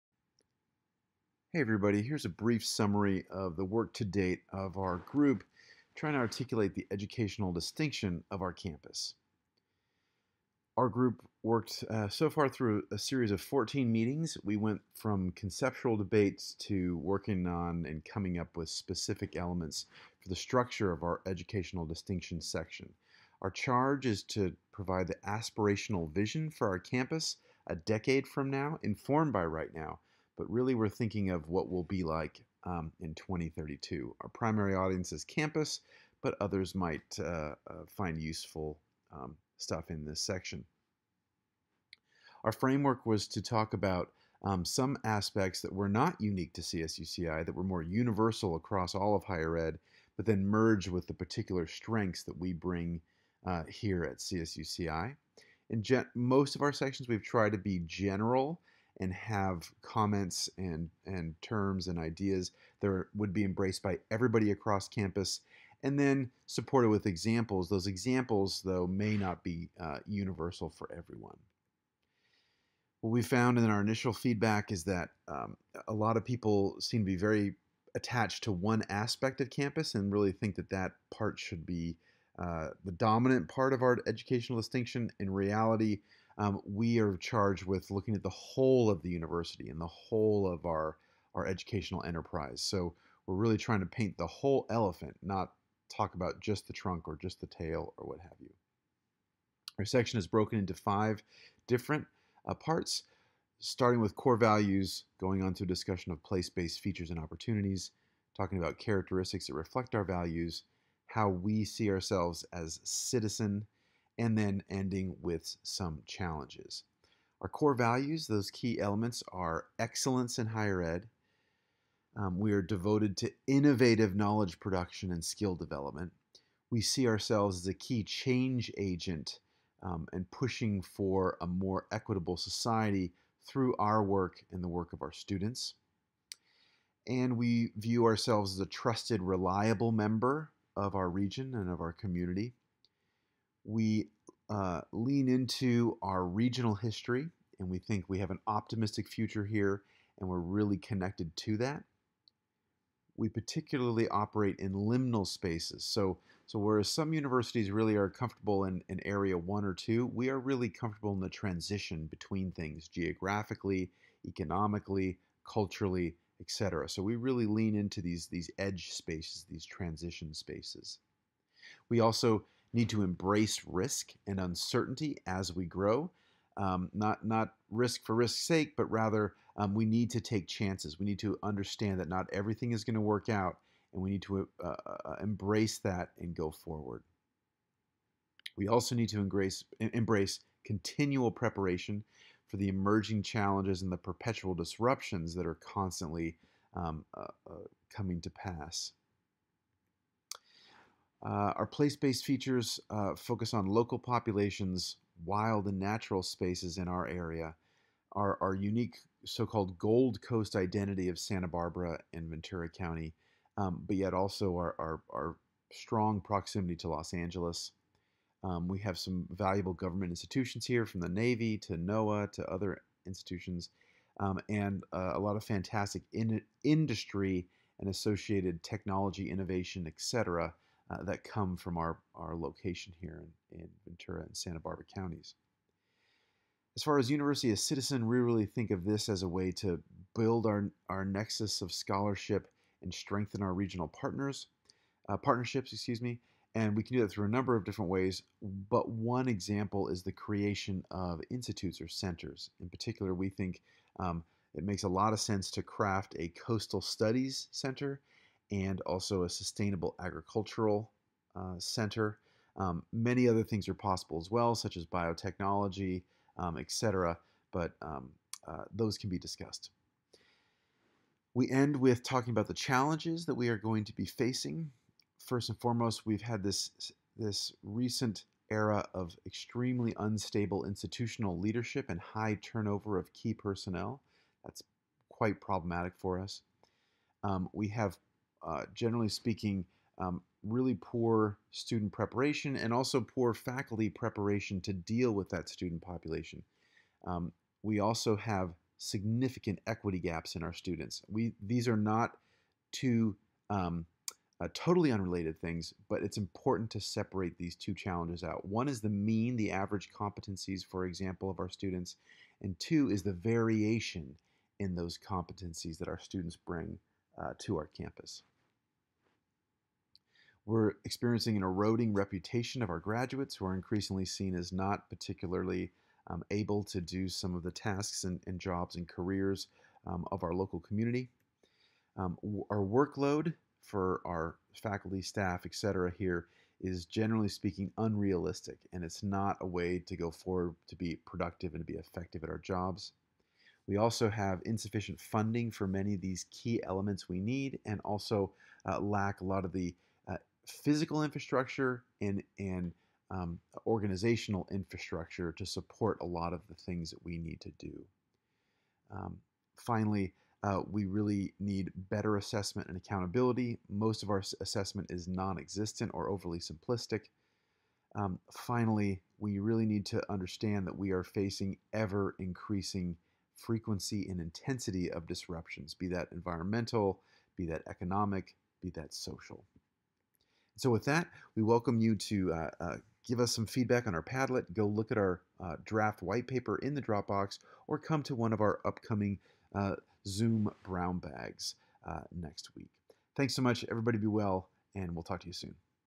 Educational Distinction Overview
This is the summary for our section of CSUCI's Charting Our Course report.